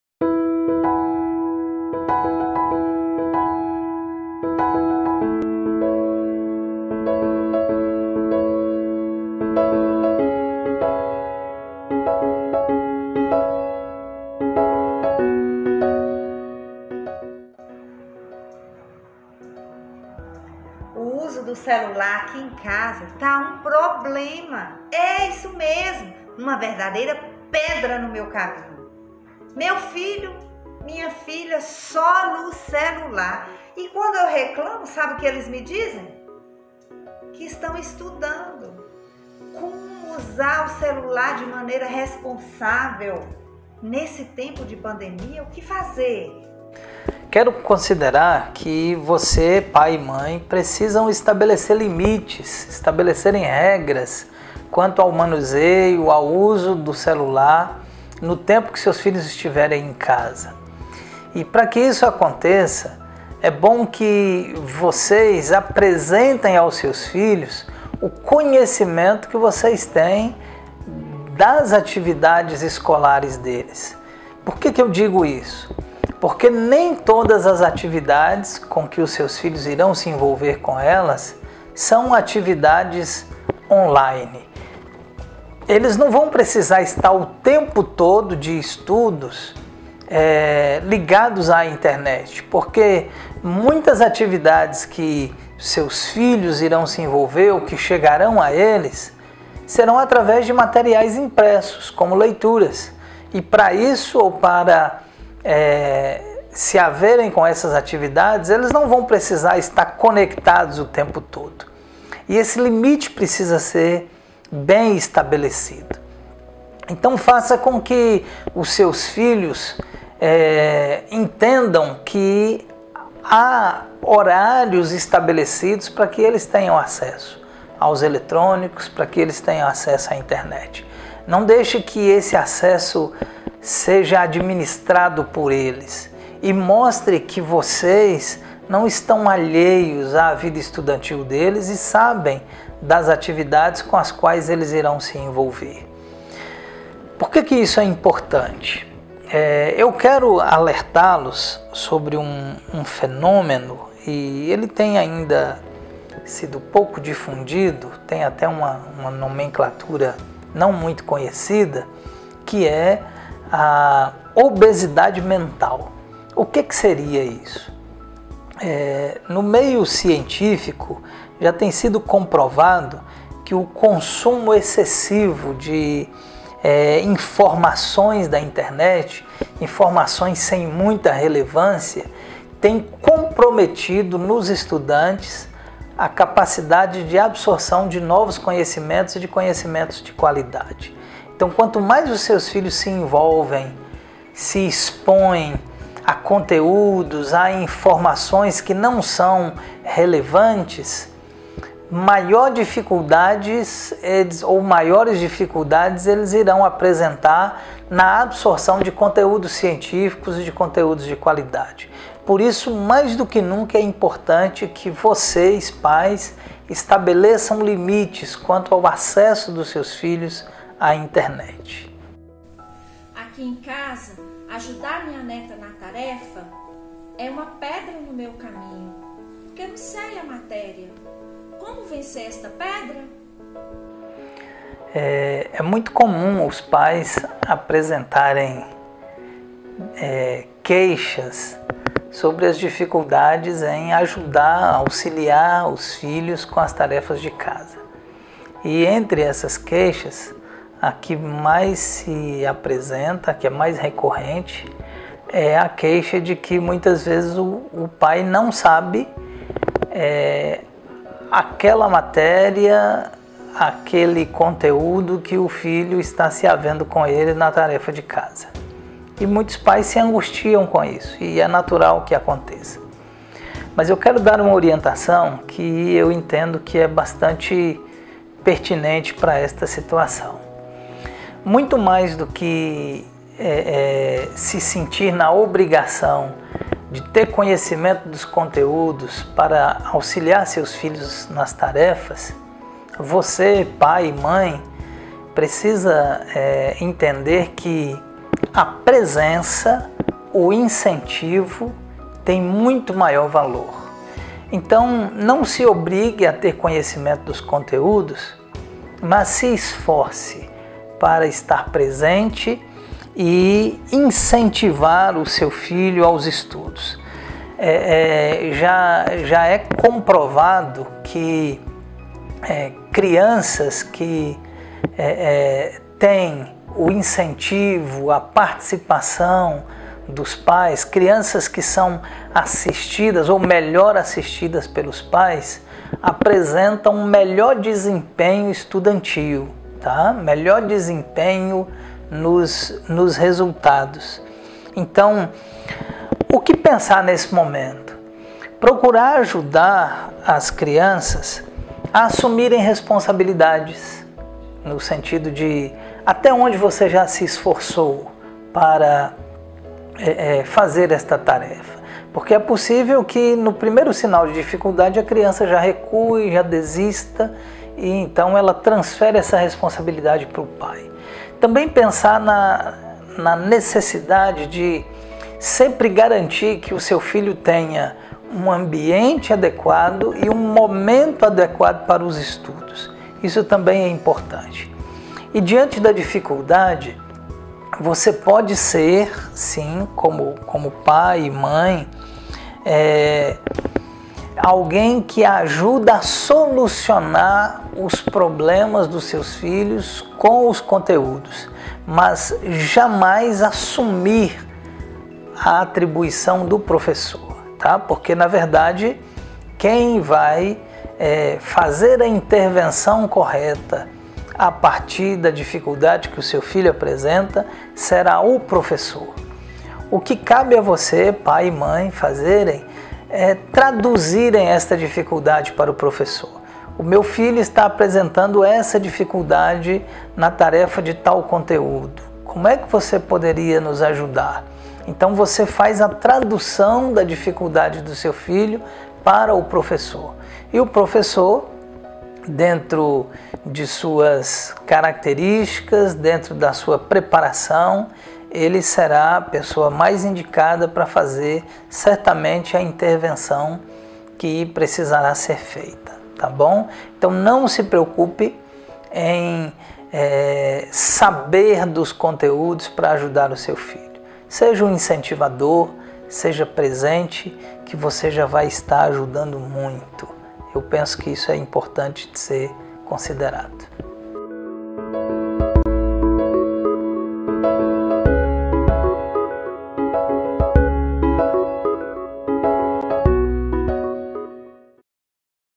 Como lidar com as pedras no caminho? "Dica profissional para pais"
Trilha da leitura junto com o psicólogo Rubens Lopes oferecem dicas profissionais a pais sobre como lidar com as pedras no caminho nestes tempos de pandemia!